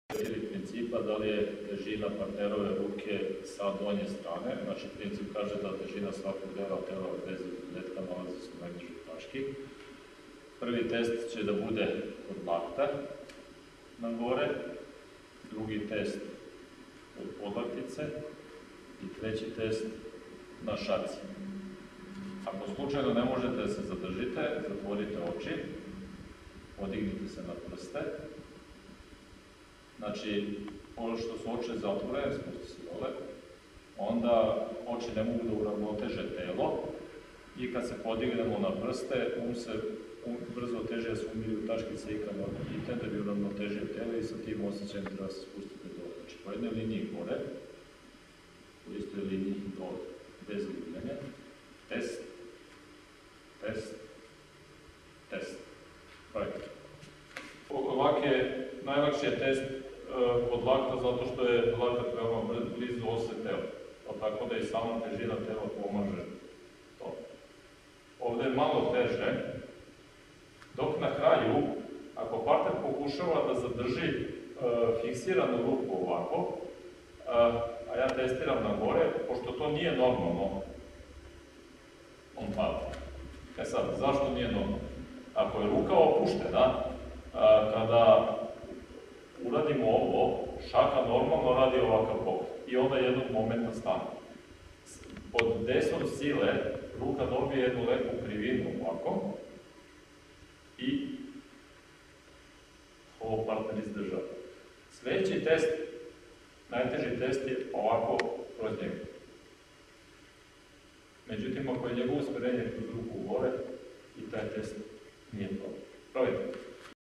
01.03 Test - stojeći položaj - ispružena ruka
Shin shin toitsu aikido klub "Beograd". Milan Jeremić objasnjava test.